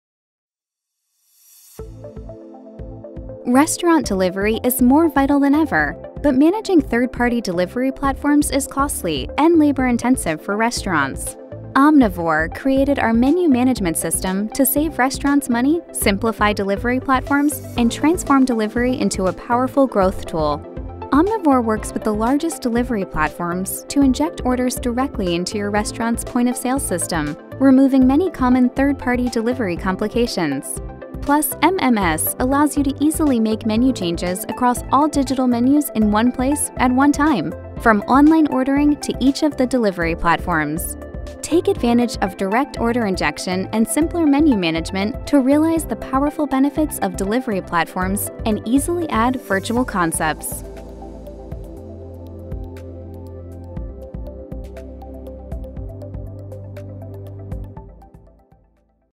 Omnivore’s Menu Management System
Omnivore’s Menu Management System simplifies UberEats, DoorDash and Postmates for restaurants. With MMS, restaurants can easily update all of their digital menus, inject delivery orders directly into the POS system, and eliminate the need for a different tablet for each delivery platform. In turn, restaurants can use delivery platforms to find new customers and increase average check size while shrinking labor costs.